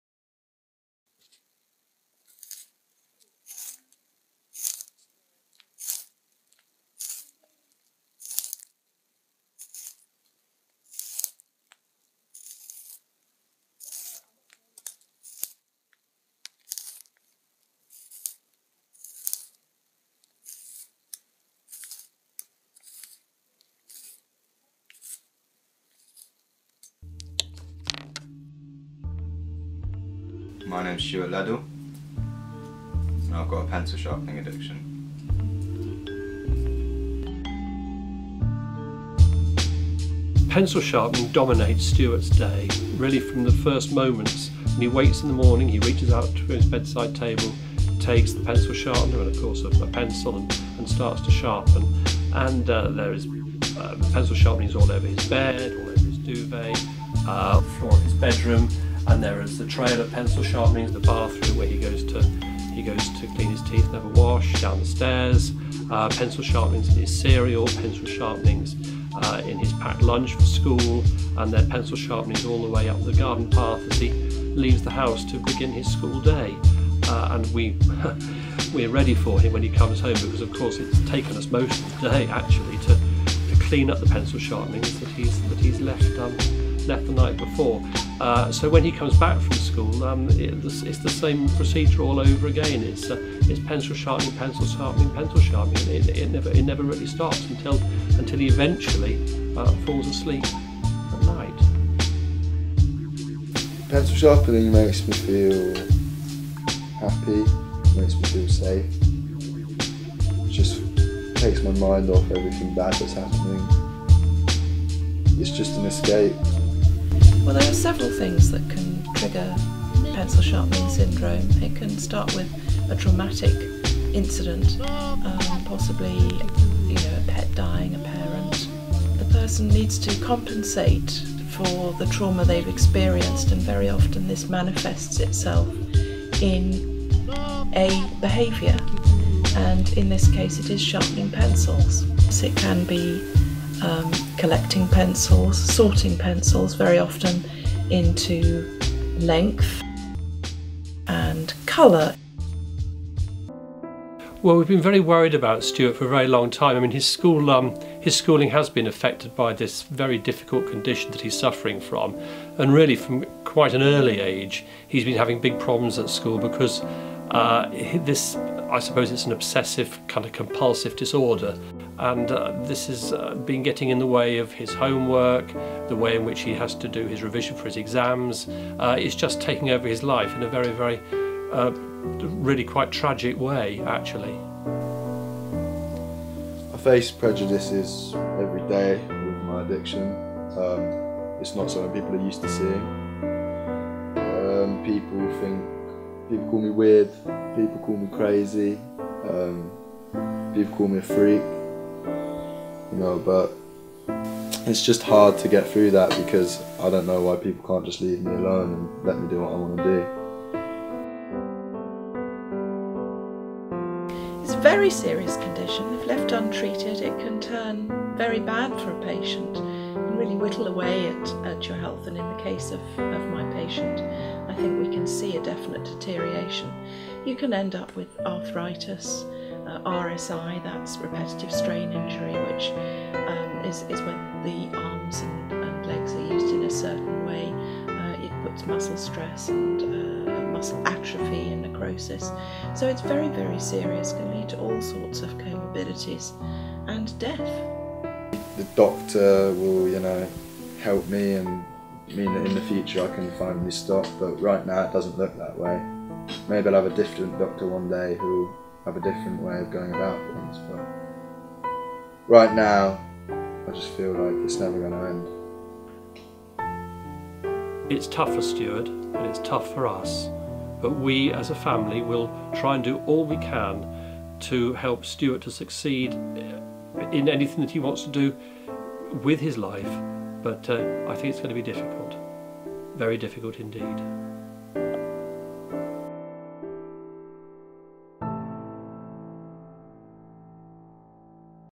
A Sharp Reality | Documentary
An insight on Pencil Sharpening Addiction. Starring Oscar Hilton